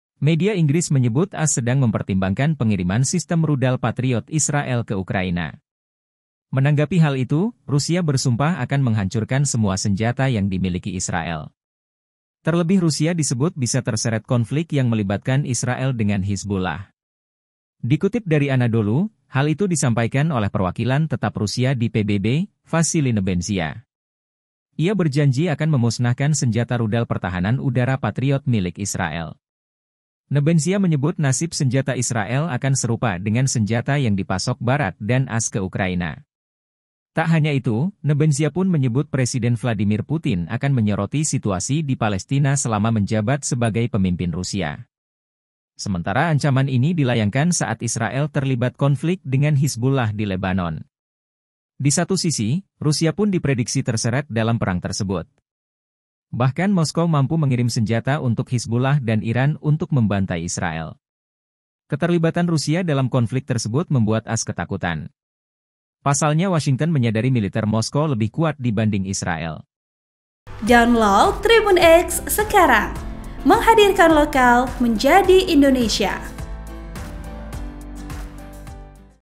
Putin Geram, Pasukan Rusia Ancam Musnahkan Semua Senjata Zionis di Tengah Perang dengan Hizbullah
SURYAMALANG.COM - Media Inggris menyebut AS sedang mempertimbangkan pengiriman sistem rudal patriot Israel ke Ukraina.

Menanggapi hal itu, Rusia bersumpah akan menghancurkan semua senjata yang dimiliki Israel.

Terlebih Rusia disebut bisa terseret konflik yang melibatkan Israel dengan Hizbullah.

Dikutip dari Anadolu, hal itu disampaikan oleh perwakilan tetap Rusia di PBB, Vassily Nebenzia.

Ia berjanji akan memusnahkan senjata rudal pertahanan udara patriot milik Israel.

Nebenzia menyebut nasib senjata Israel akan serupa dengan senjata yang dipasok Barat dan AS ke Ukraina.

"Nasib senjata yang pada akhirnya akan diekspor ke Ukraina dari mana pun sudah jelas. Senjata-senjata itu akan dihancurkan seperti senjata lain yang dipasok Barat dan AS ke Ukraina," katanya dalam konferensi pers.

Tak hanya itu, Nebenzia pun menyebut Presiden Vladimir Putin akan menyoroti situasi di Palestina selama menjabat sebagai pemimpin Rusia.

Sementara ancaman ini dilayangkan saat Israel terlibat konflik dengan Hizbullah di Lebanon.

Di satu sisi, Rusia pun diprediksi terseret dalam perang tersebut.

Bahkan Moskow mampu mengirim senjata untuk Hizbullah dan Iran untuk membantai Israel.

Keterlibatan Rusia dalam konflik tersebut membuat AS ketakutan.

Pasalnya Washington menyadari militer Moskow lebih kuat dibanding Israel.

(Tribun-Video.com)